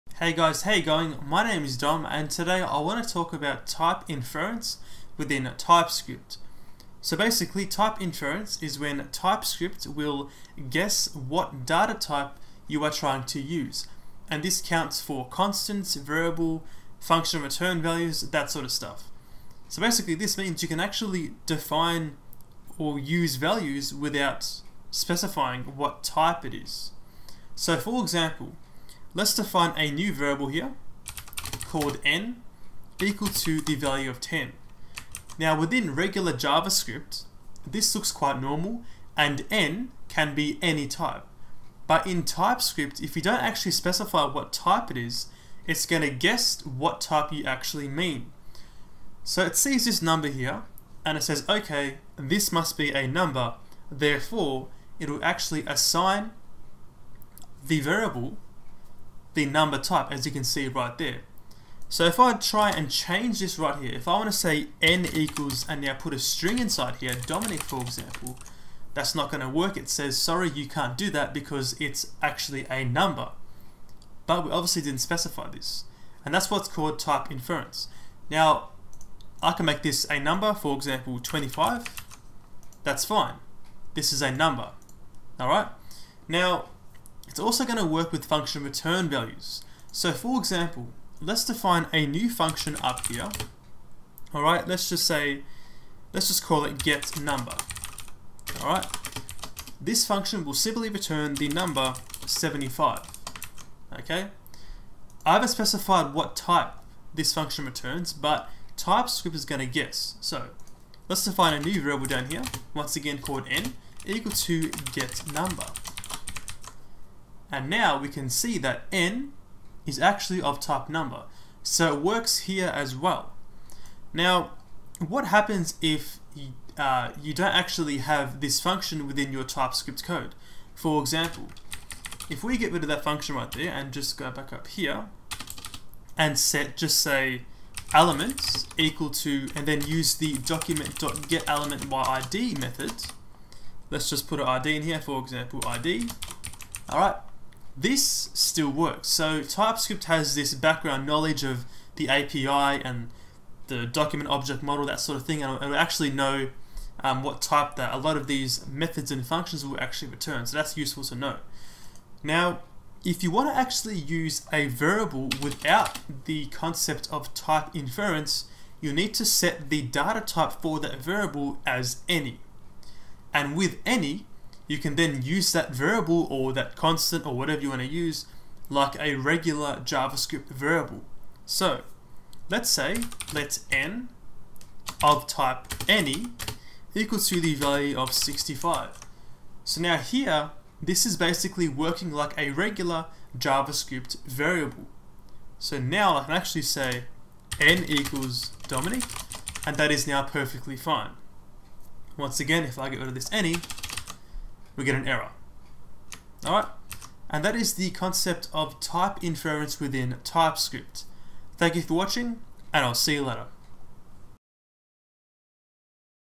Type Inference - TypeScript Programming Tutorial #4
Type Inference is when TypeScript will guess the data type of an expression - this includes things such as variables, constants and function return values.

In this video we explore this paradigm and how it is used in TypeScript, and also when you would like to specify a variable with any type rather than the default type inference.